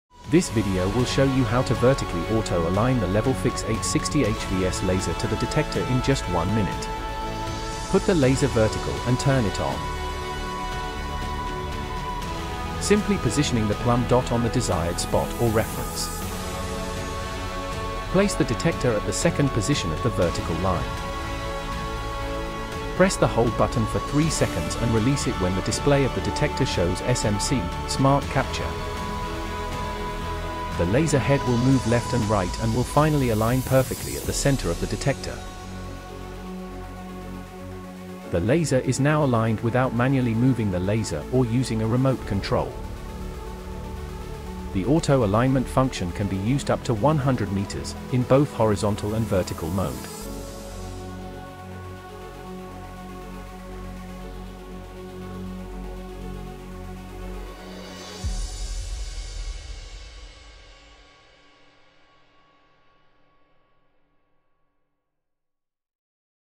How to align the LEVELFIX 860HVS with the auto tracking function
The 860HVS is equipped with a handy auto-align function. This function ensures that the laser automatically locates the center of the receiver. This saves time and money. In this video we briefly explain how that works.